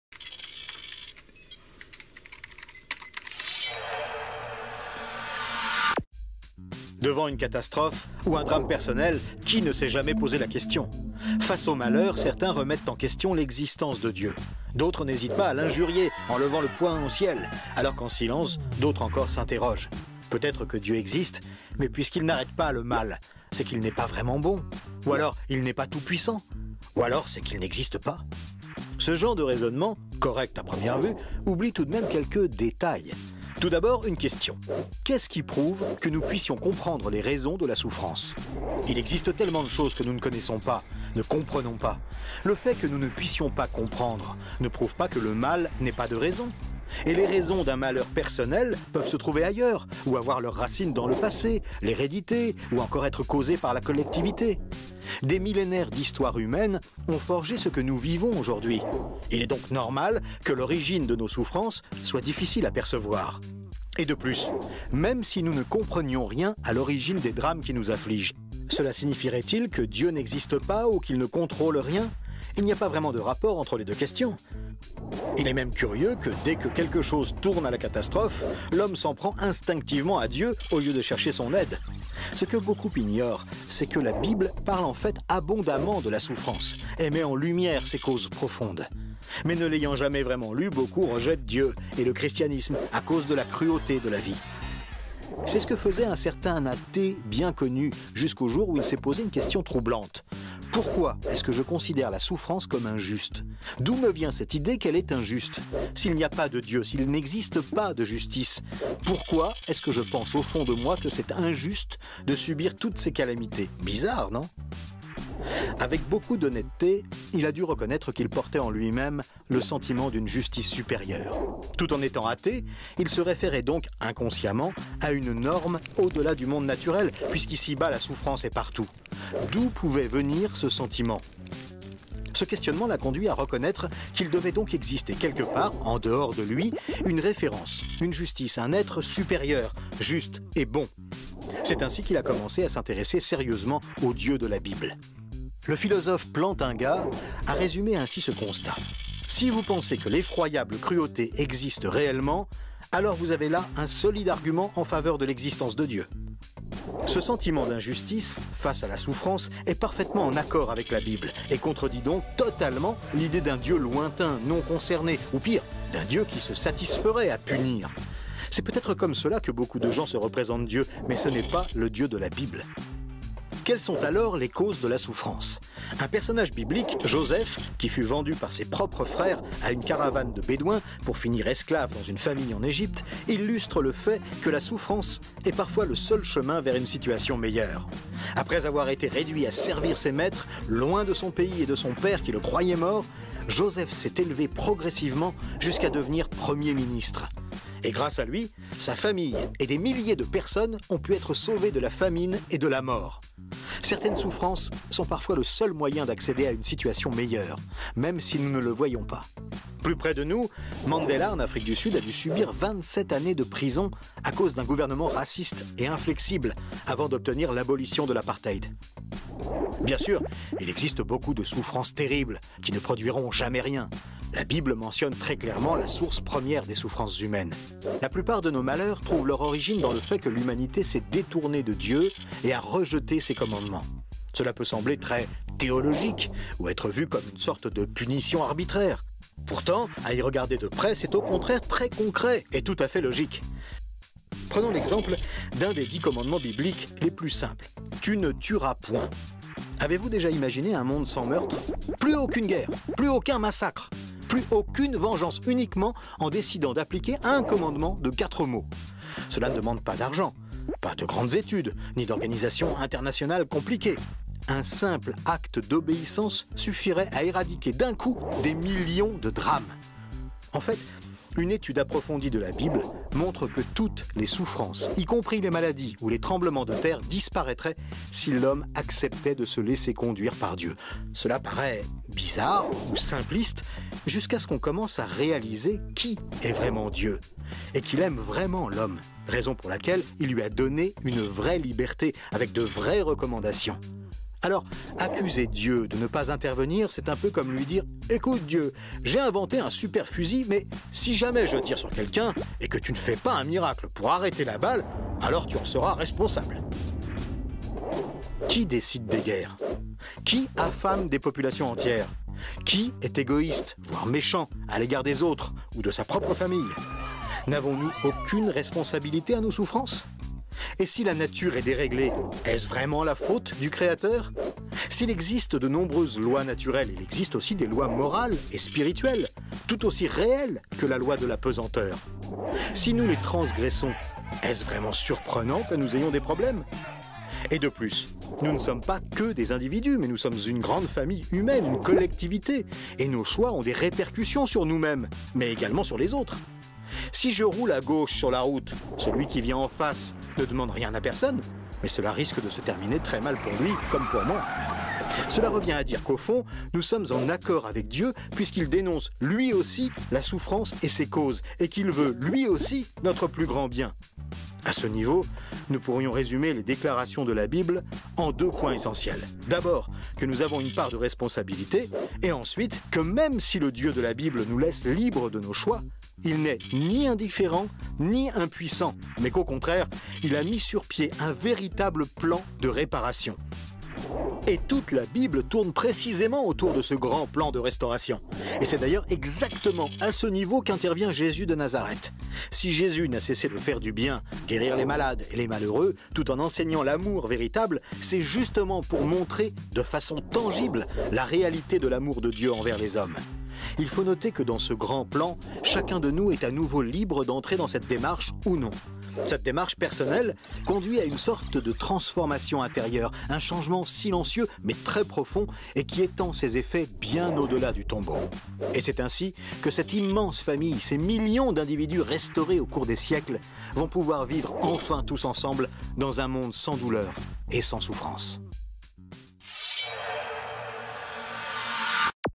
➢ Si Dieu existe vraiment alors pourquoi souffrons-nous ?
Cette vidéo clarifie cette grande question que tant d'hommes et de femmes se sont posées et se posent depuis toujours. La Bible apporte de vraies réponses, qui demandent cependant une vraie honnêteté et une certaine humilité pour être acceptées. Bien plus, cette question qui taraude l'humanité, a amené des millions d'hommes et de femmes à découvrir la fabuleuse réponse divine. Et en définitive, la seule véritable réponse, totale, concrète et durable à toutes nos souffrances ! Une analyse honnête et un message d'espoir authentique.